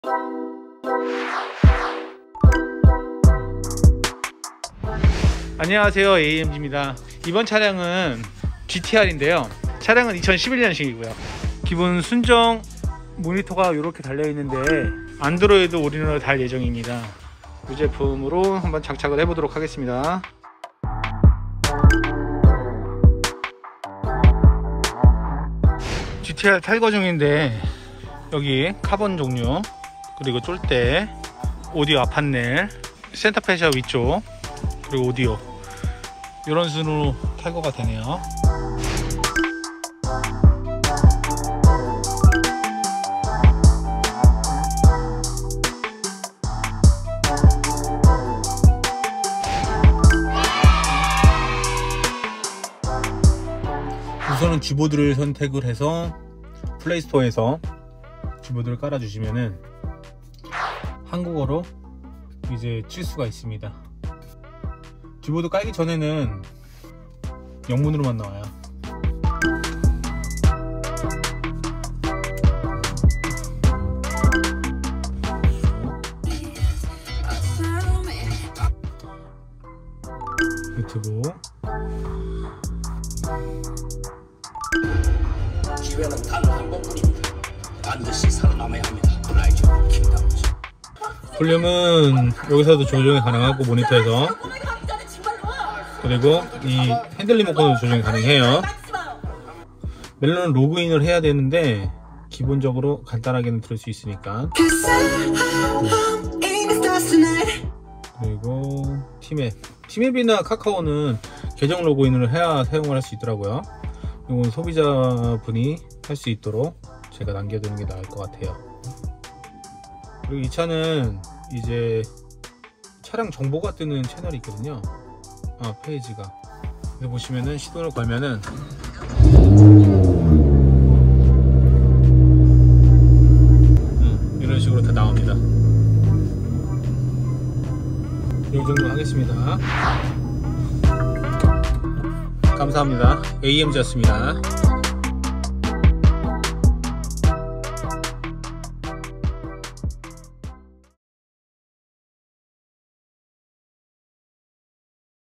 우와~2011 닛산 GTR / 안드로이드올인원으로 달라진 GTR [부천AMG]
국산차, 수입차 이제 필수가 되어가는 안드로이드 올인원

스마트한 시대 내 차에 안드로이드 올인원 한번 달아보세요

보유 중이거나 구매해서 가져오시는 제품도 언제든 장착 가능합니다.